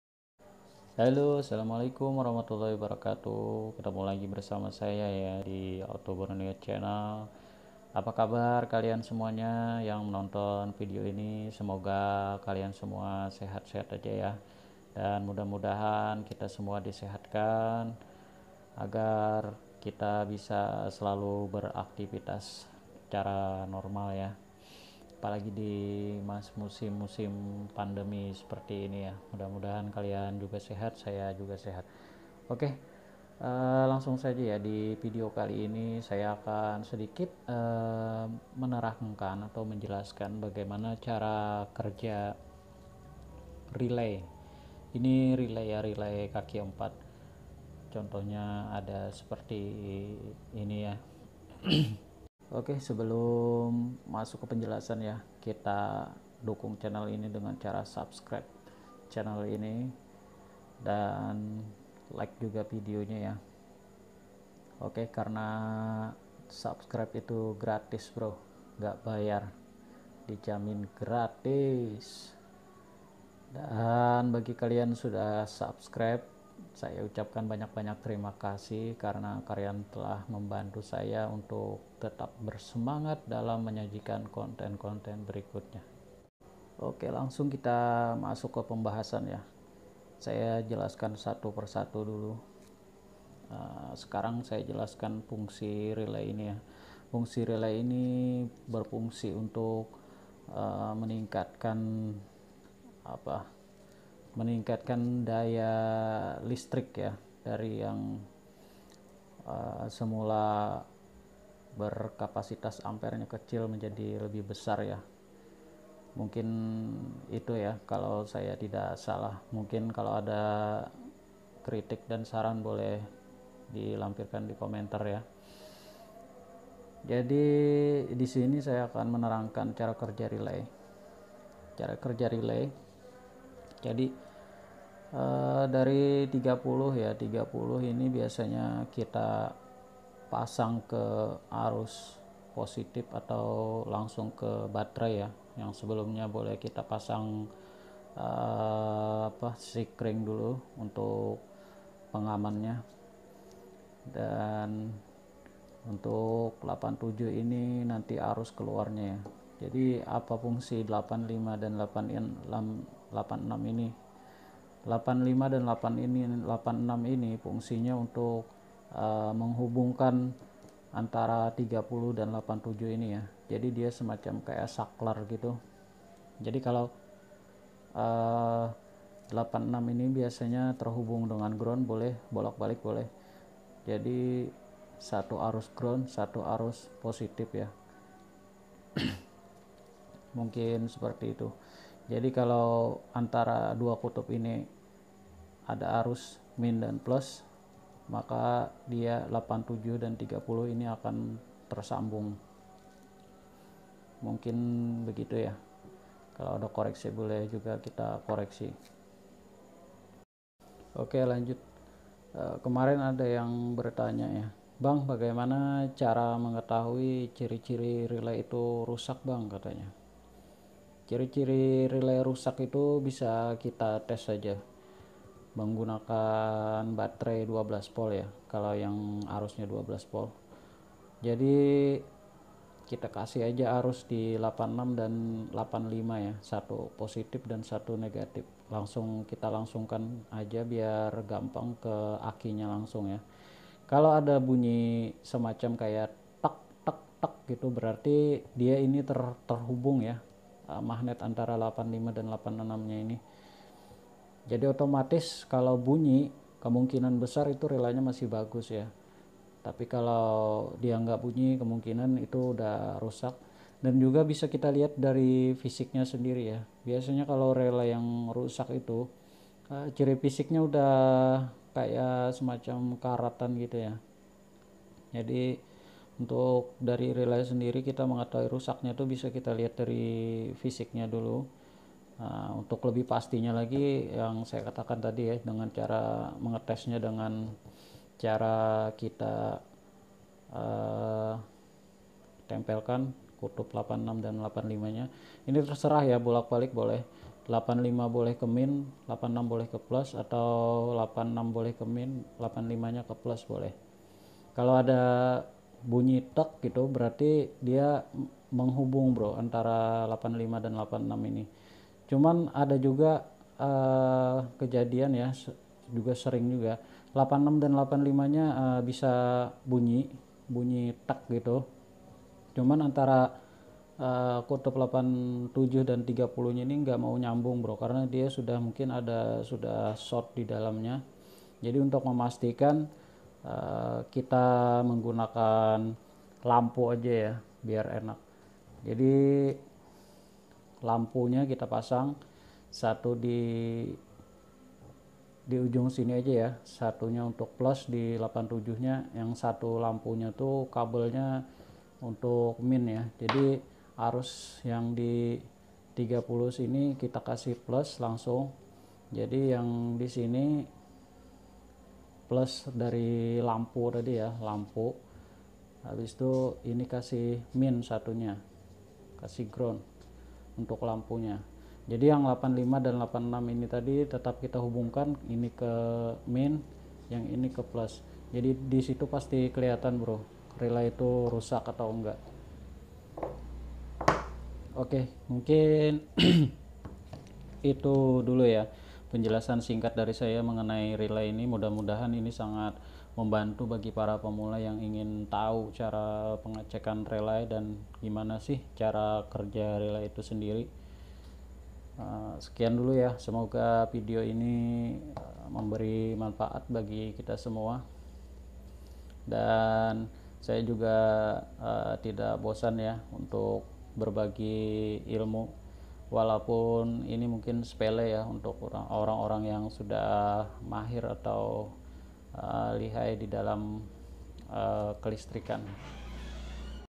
CARA CEK RELAY 4 KAKI
mungkin masih banyak dari kalian yg belum begitu faham cara kerja relay dan bagaimana cara cek relay yang rusak,divideo ini saya menjelaskan secara tertulis cara cek relay yang rusak,